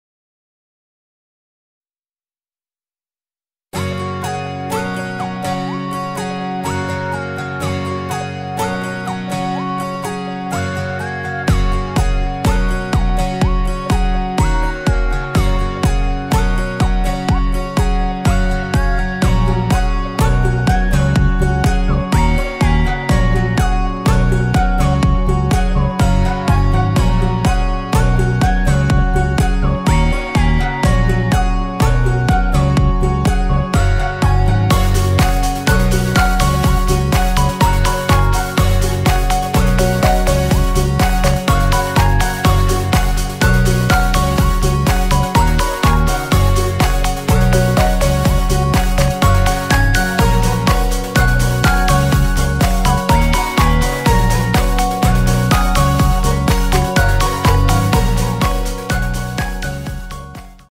HEAT DETECTION TAPE AS-92【NiGK Corporation】
This movie is about the roll type indicator “HEAT DETECTION TAPE”.

Manufacturer; A-SPE EUROPE SP. J.